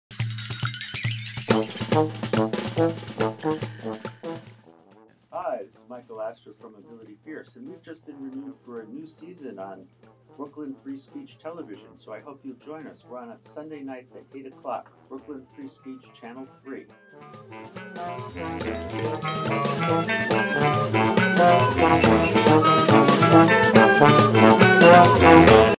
License Renewed
Ability Fierce has been renewed for a third season on Brooklyn Free Speech TV Channel 3. You can catch it Sunday nights at 8:00 p.m. (or Monday mornings at 3 a.m. if you're so inclined). We post all the episodes on our YouTube Channel but you can also see it on cable TV at:

In Brooklyn on:
Spectrum 1996
Optimum 69
RCN 84
Across NYC:
Verizon 44
Around the globe (via live stream):
BRICartsmeida.org/3